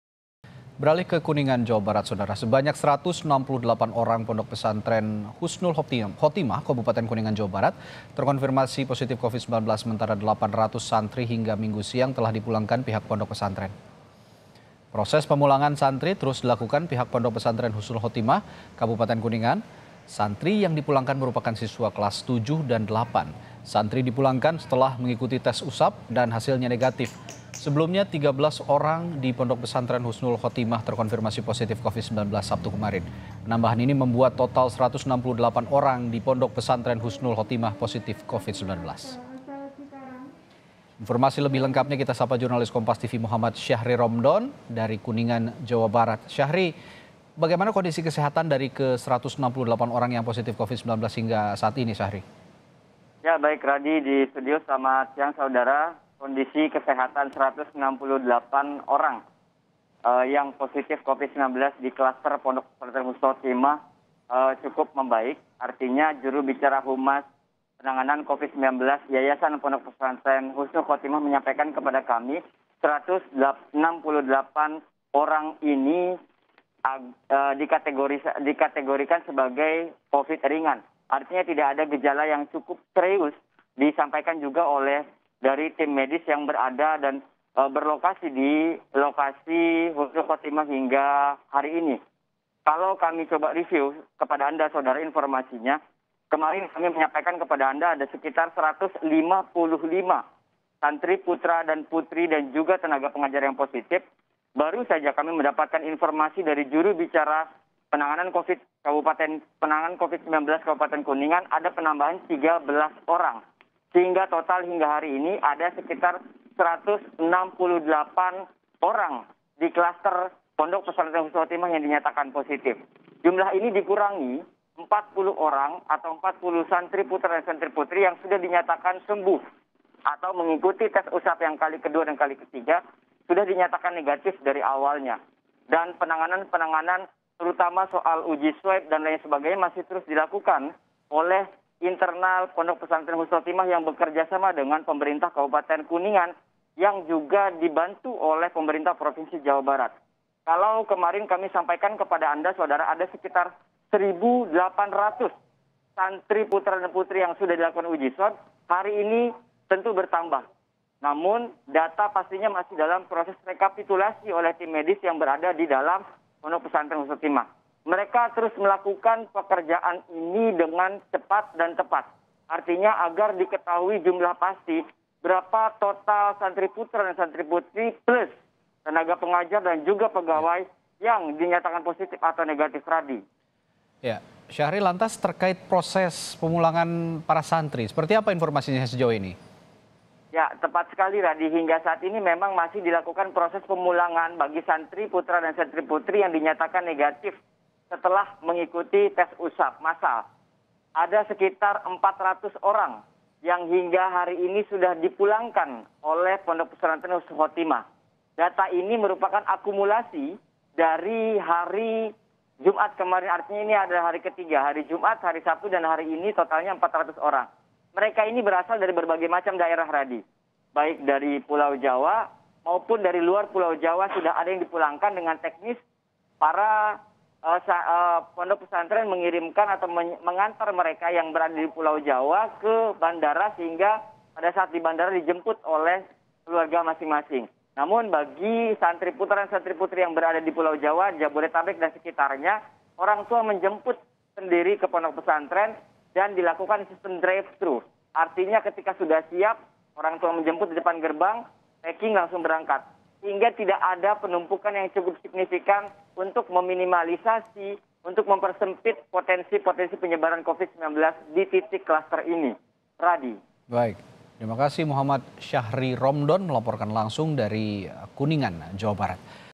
800 Santri Dipulangkan Dari Pondok Pesantren Demi Cegah Penyebaran Corona
KUNINGAN, KOMPAS.TV - Sebanyak 168 orang santri di Pondok Pesantren Husnul Khotimah, Kabupaten Kuningan, Jawa Barat terkonfirmasi positif covid-19.

Sementara, 800 santri hingga hari Minggu (04/10) siang telah dipulangkan pihak pondok pesantren.

Proses pemulangan santri terus dilakukan pihak pondok pesantren Husnul Khotimah, Kabupaten Kuningan.

Santri yang dipulangkan merupakan siswa kelas 7 dan kelas 8.

Santri dipulangkan setelah mengikuti tes usap dan hasilnya negatif.

Sebelumnya, 13 orang di Pondok Pesantren Husnul Khotimah terkonfirmasi positif covid-19 pada hari Sabtu kemarin.

Penambahan ini membuat total 168 orang di Pondok Pesantren Husnul Khotimah positif covid-19.